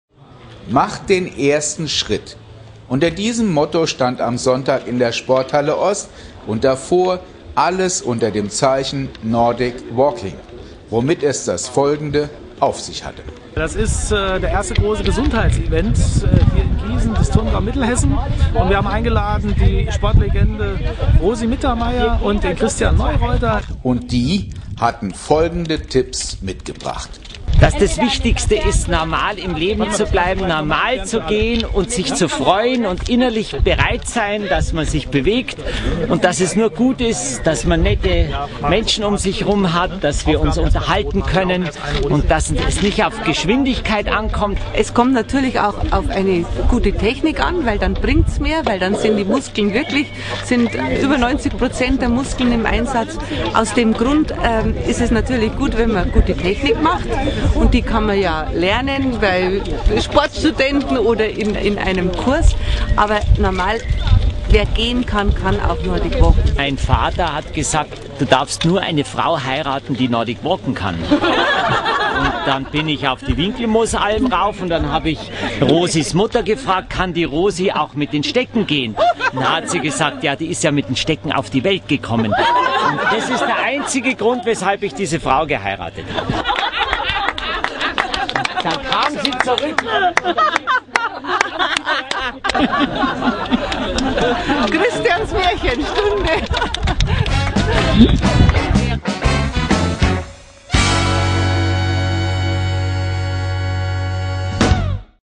Nordic-Walking-Day mit Rosi Mittermaier & Christian Neureuther
Unter dem Motto "Mach den ersten Schritt" hatte der Turngau Mittelhessen zu seinem Gesundheits-Event 2010 in die Sporthalle Ost geladen. Turngau-Vorsitzender Dr. Dennys Sawellion präsentierte stolz die Sportlegenden aus Garmisch.